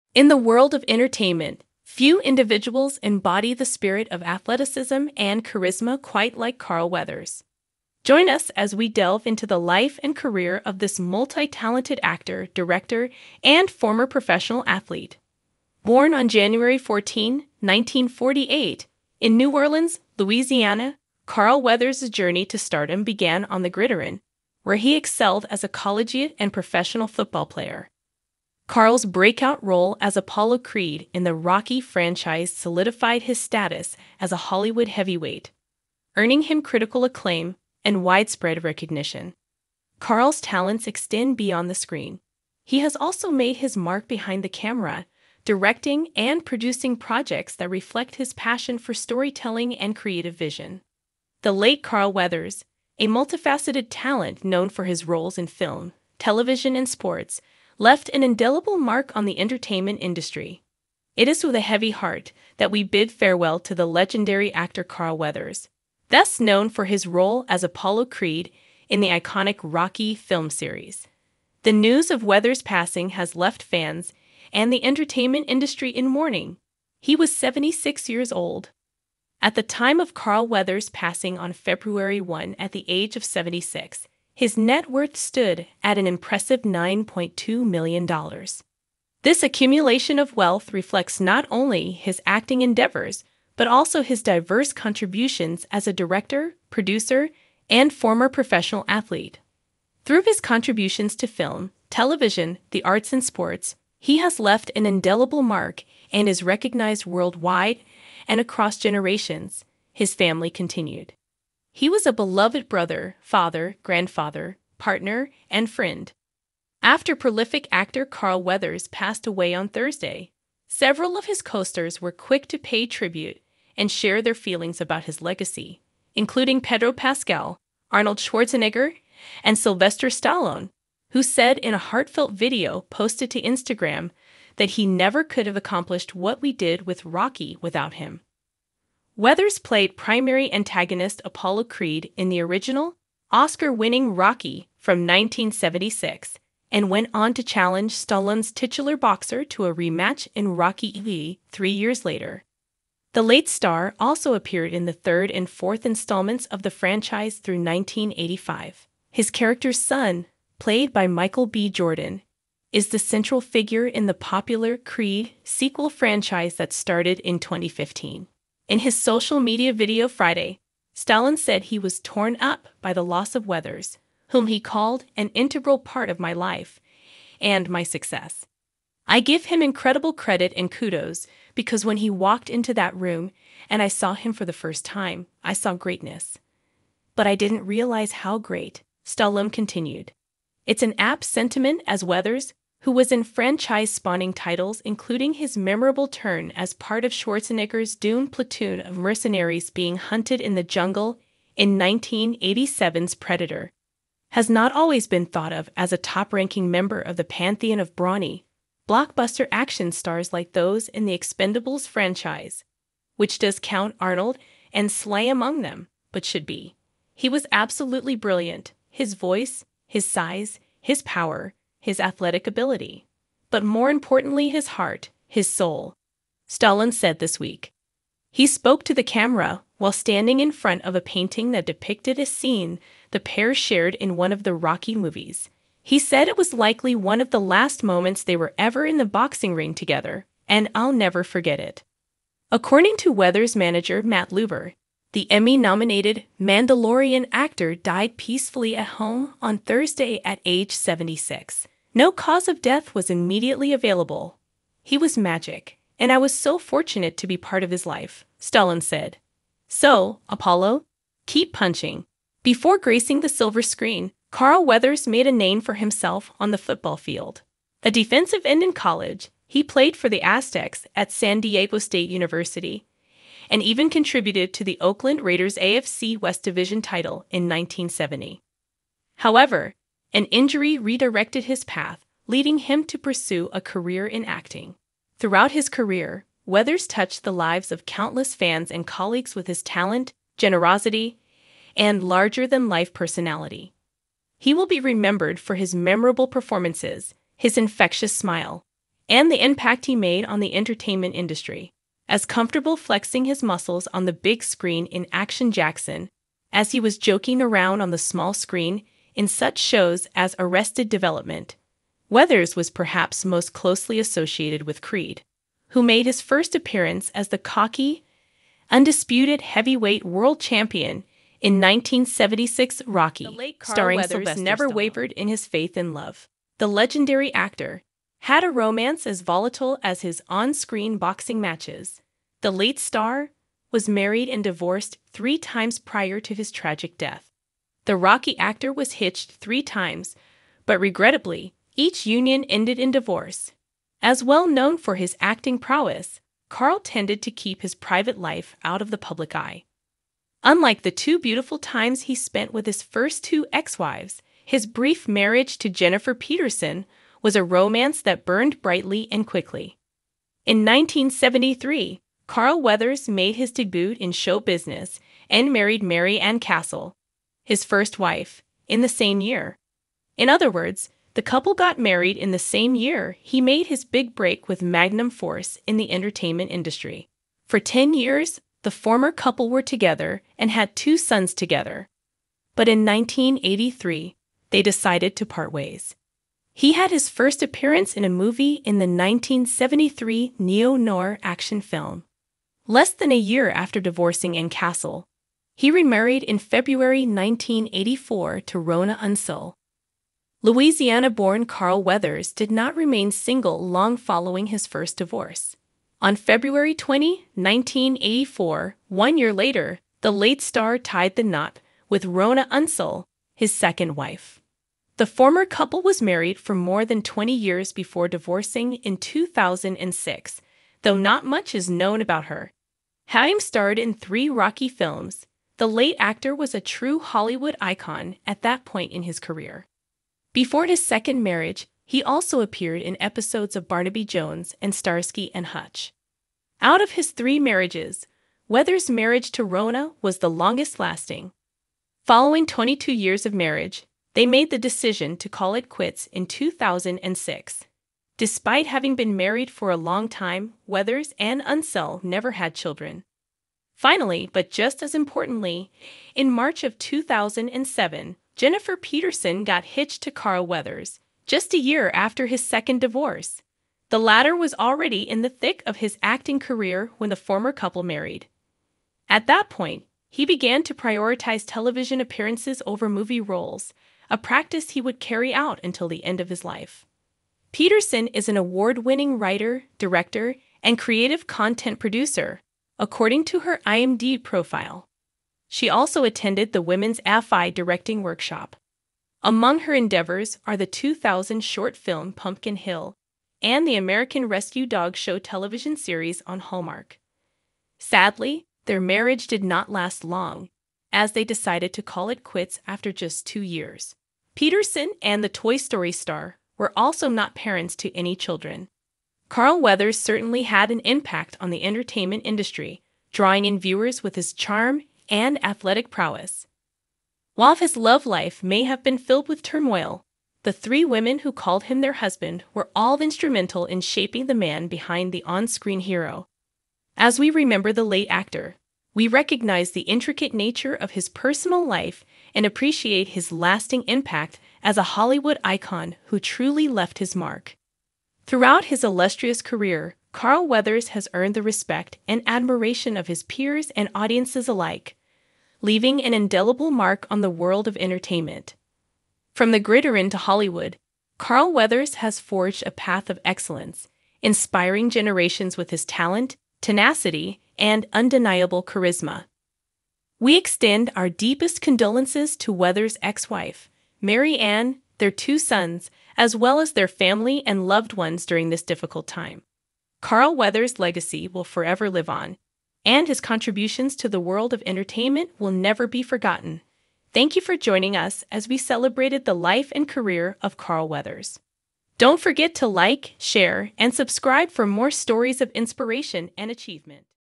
Inside Messy Life Of ‘Rocky’s’ “Apollo Creed” Actor, Carl Weathers, Net Worth & D3ath At 76
Carl Weathers certainly had an impact on the entertainment industry, drawing in viewers with his charm and athletic prowess. While his love life may have been filled with turmoil, the three women who called him their husband were all instrumental in shaping the man behind the on-screen hero.